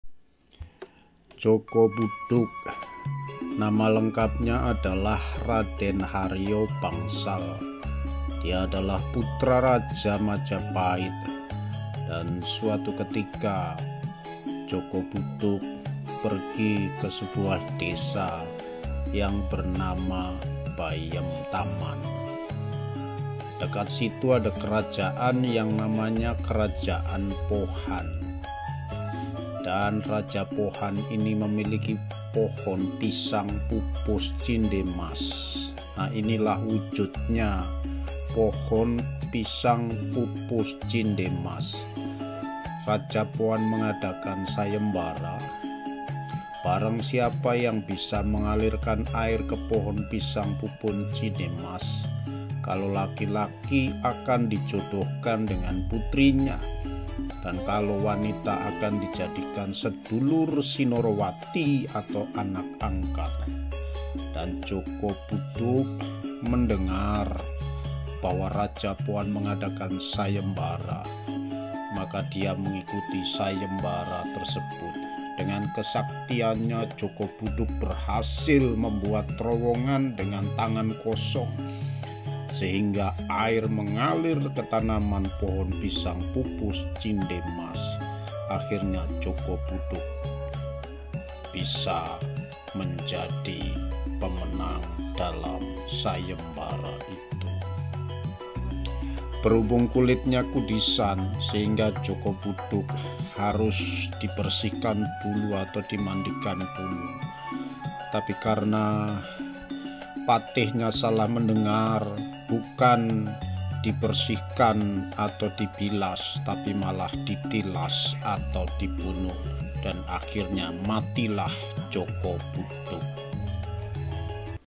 Pisang Pupus Cinde Mas
Kisah tentang Joko Budug atau nama asli Haryo bangsal anak Raja Majapahit yang memiliki wajah tampan namun sang Raja ingin agar Haryo Bangsal segera memiliki Istri,namun Haryo bangsal blm mau memiliki istri karena masih mau mengejar ilmu,dan akhirnyanpergi dari keraton dan sampailah di desa bayem taman,di dekat desa itu ada Kerajaan Pohan,dan Raja Pohan Memiliki Pohon Pisang Pupus Cinde Mas,karena tidak mau pohon pisangnya layu maka raja membuat sayembara,barang siapa yang bisa mengalirkan Air sampai ke Pohon Pisang maka kalau laki2 akan jadikan Menantu tapi kalunperempuan di jadikan saudara perempuan,dan akhirnya Joko Budung mengikuti sayembara tersebut dan dengan kesaktiannya joko Buduq dapat membuat terowongan dengan tangannya, sehingga Joko Budug keluar jadi Pemenang,dst